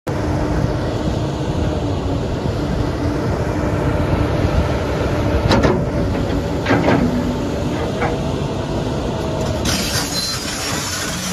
2004 KOMATSU PC308US LC-3 For Sale
Very good machine. Tight, with no leaks. A/C blows cold. 55" bucket. Other bucket sizes available.